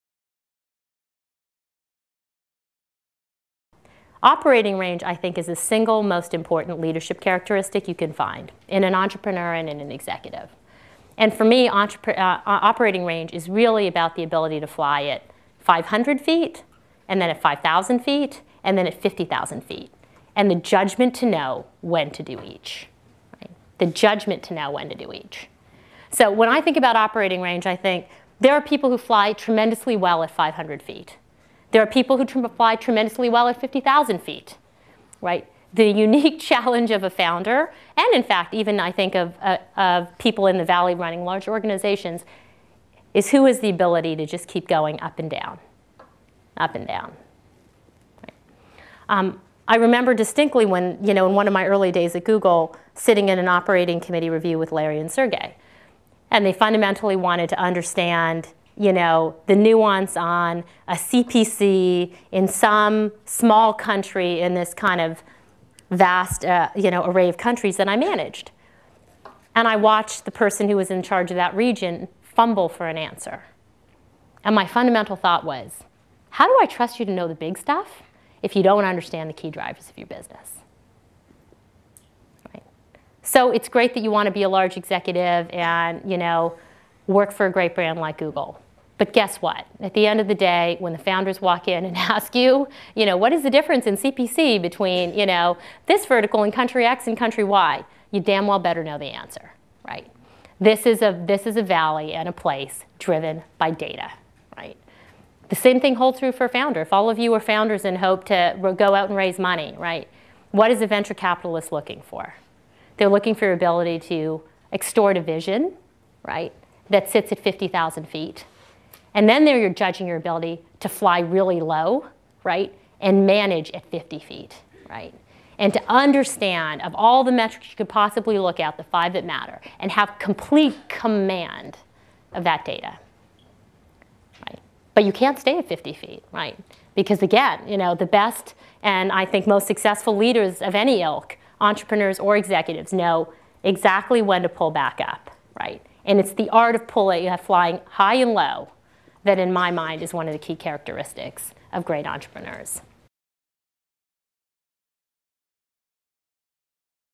Sukhinder Singh Cassidy: Develop Operating Range
Entrepreneur Sukhinder Singh Cassidy argues the judgment to know at what level to examine your business at any given moment is a vital leadership quality. Using a short anecdote from her time at Google, Singh Cassidy explains successful founders and executives must understand the tactical drivers of a business to effectively manage from the top of an organization.